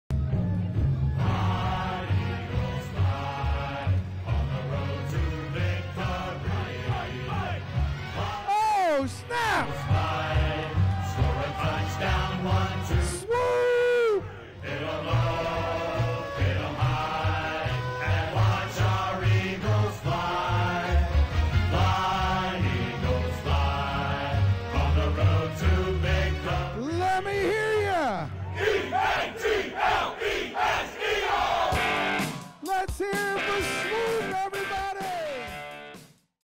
Bride Surprises Groom with Swoop the Eagles Mascot {Fallon & Victor}
Our bride Fallon surprised her husband Victor with a visit from Swoop the Eagles Mascot during their wedding reception at Scotland Run. We can't wait to share their wedding film. Thanks for letting us be a part of this awesome event!